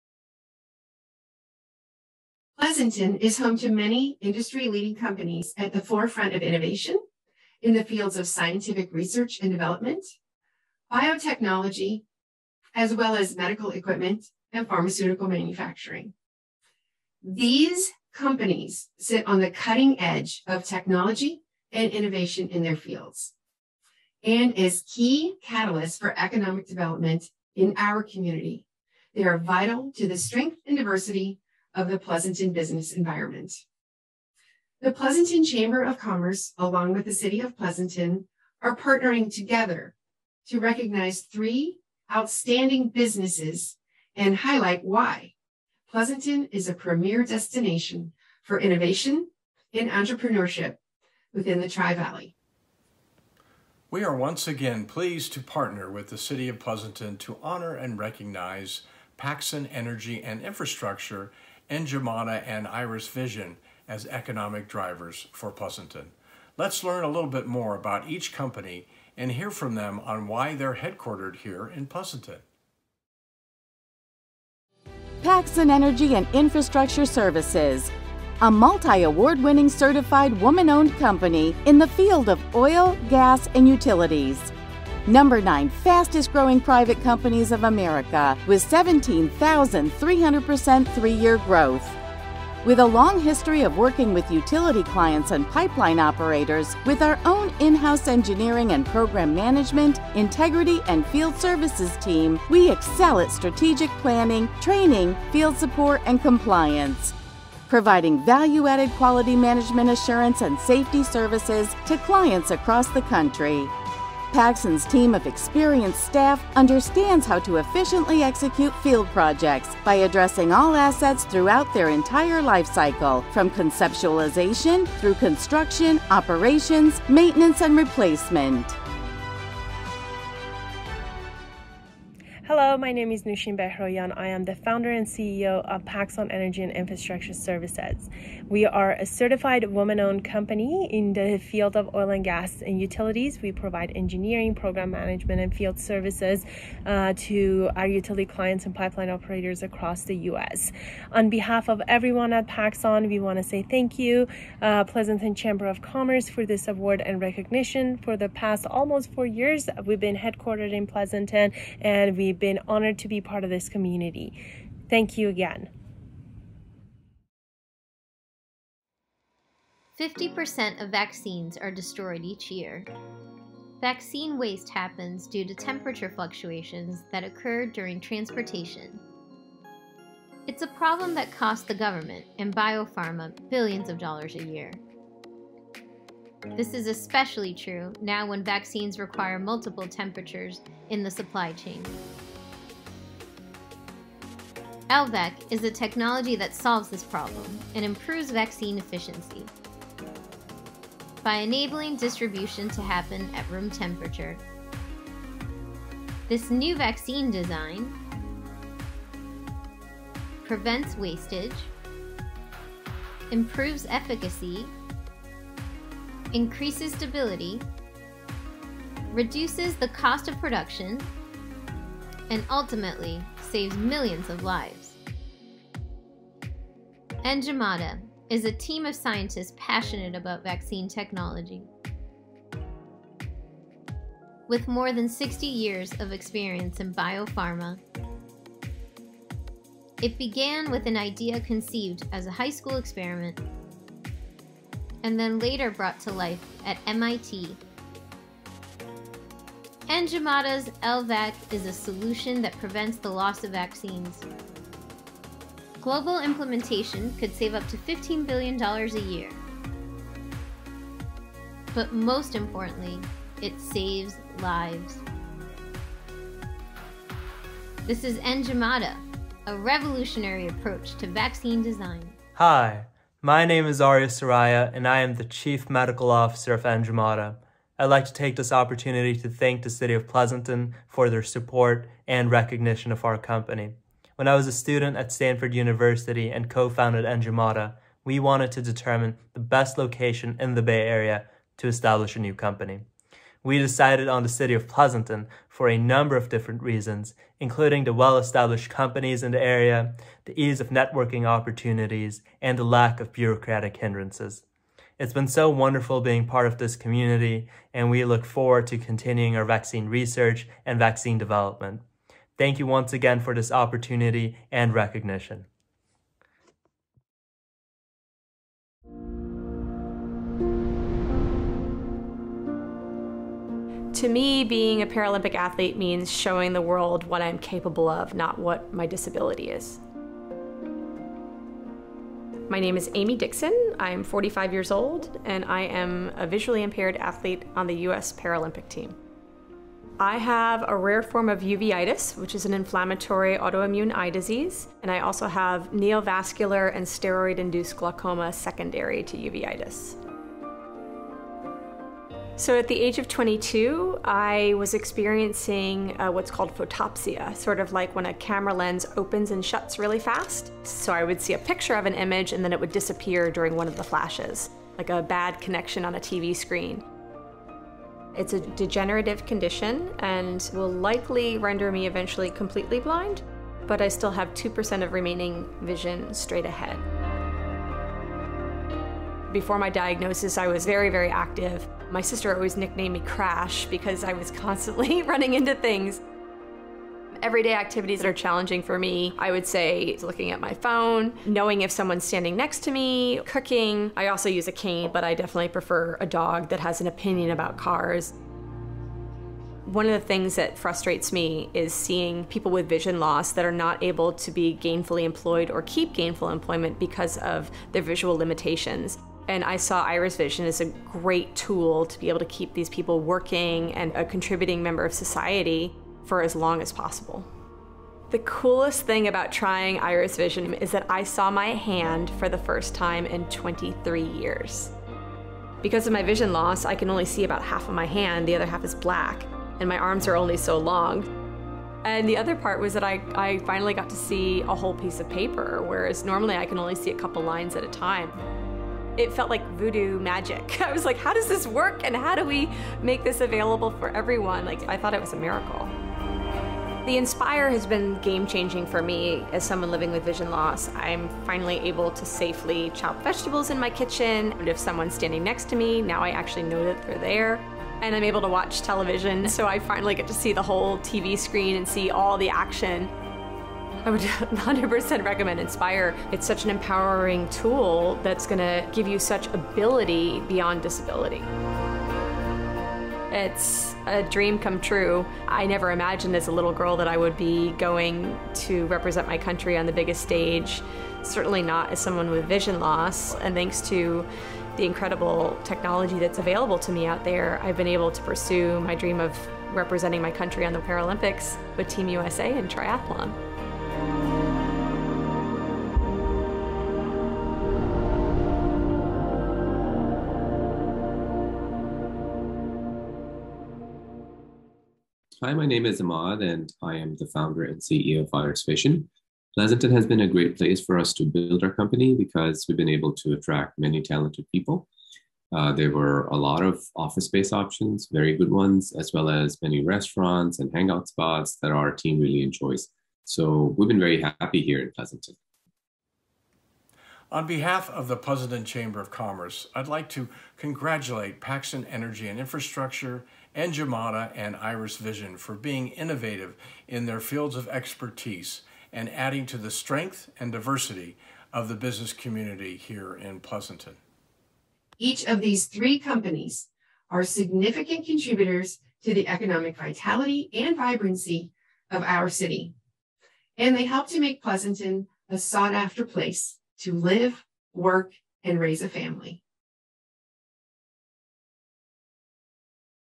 2021 Pleasanton Economic Drivers
Promoting Local Business Through Innovation. The Pleasanton Chamber of Commerce and the City of Pleasanton are partnering to recognize three successful businesses and highlight Pleasanton as the premier destination for innovation and entrepreneurship in the Tri-Valley. The dynamic Pleasanton-based companies that are being recognized in 2021 are Enigmata, IrisVision Global and Paxon Energy & Infrastructure.